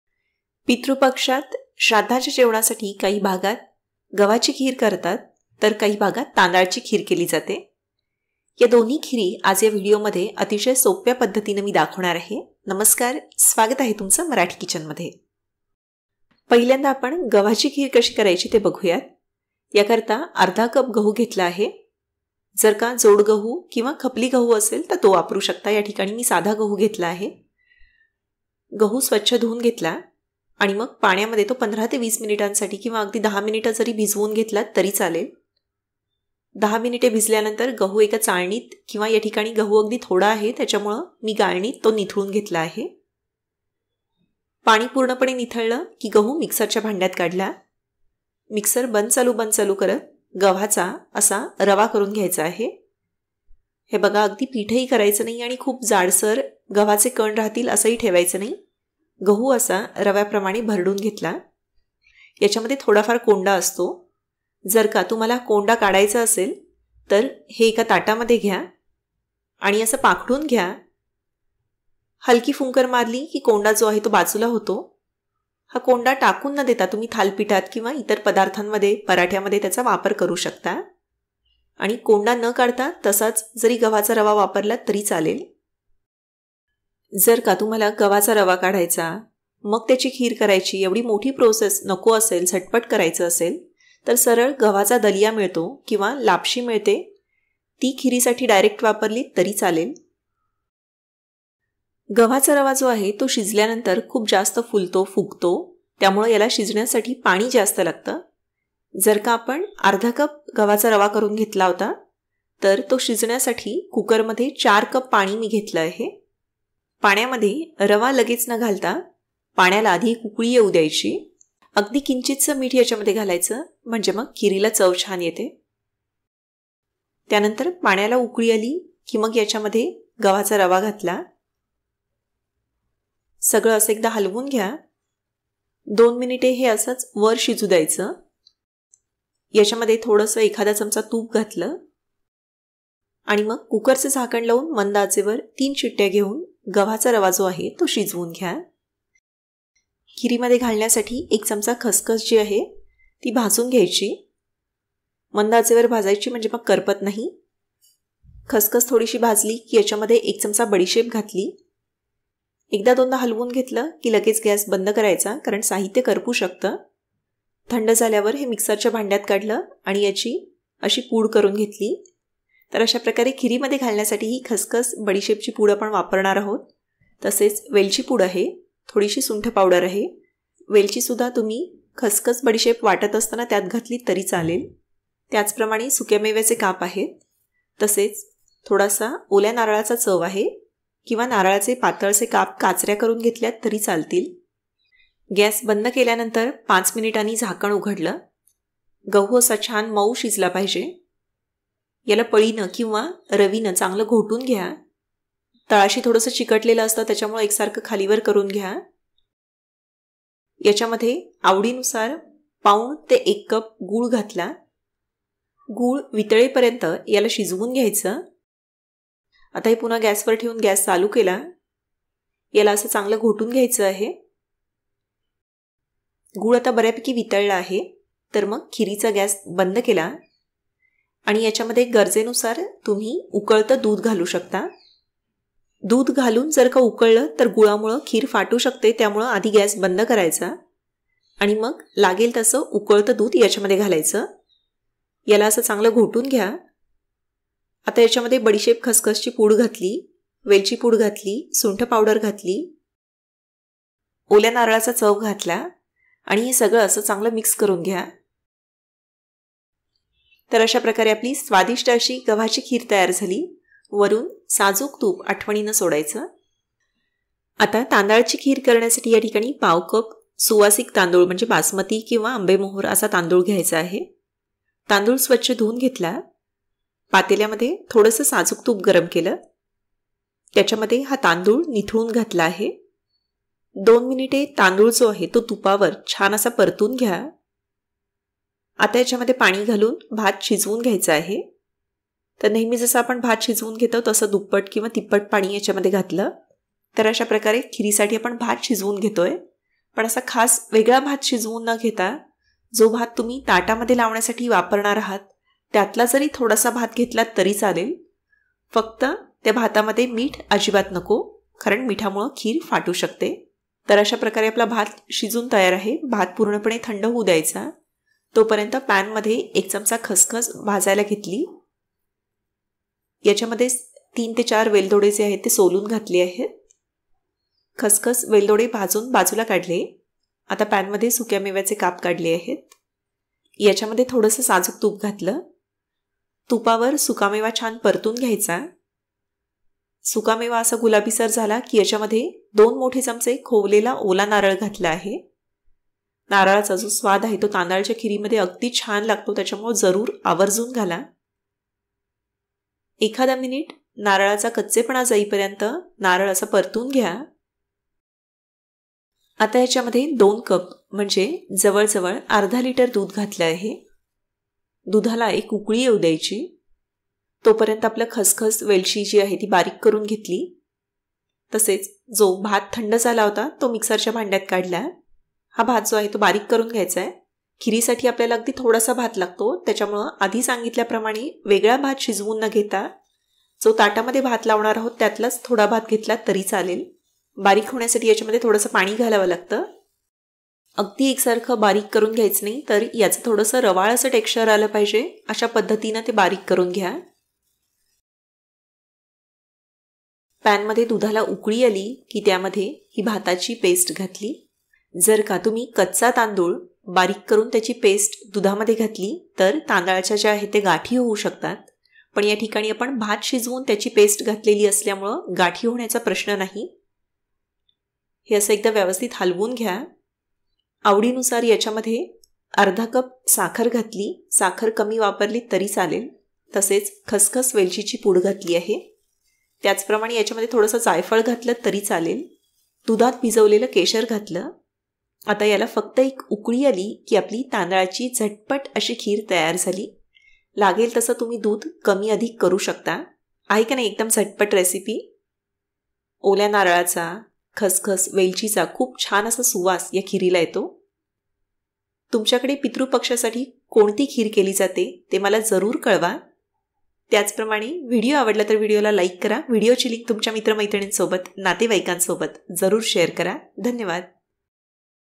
पितृपक्ष विशेष खिरीचे २ झटपट प्रकार, तांदळाची खीर आणि गव्हाची खीर | Rice Kheer | Gavhachi Kheer
गव्हाची खीर
साहित्य
१/२ कप गहू
४ कप पाणी
किंचित मीठ
साजूक तूप ३ शिट्ट्या
१ कप गूळ
दूध
१ चमचा खसखस + १ चमचा बडीशेप
खसखस बडीशेप पूड
वेलची +सुंठ पावडर
सुकामेवा उकळते गॅस बंद करा
ओल्या नारळाचा चव

तांदळाची खीर
१/4 कप तांदूळ
१/२ लिटर दूध
साखर
२ चमचे साजूक तूप
ओल्या नारळाचा चव
सुकामेव्याचे काप
१ चमचा खसखस
जायफळ वेलची पूड
केशर
Dosa Tawa
Mixing bowl
Grill Toaster
Non-stick fry pan
Appe pan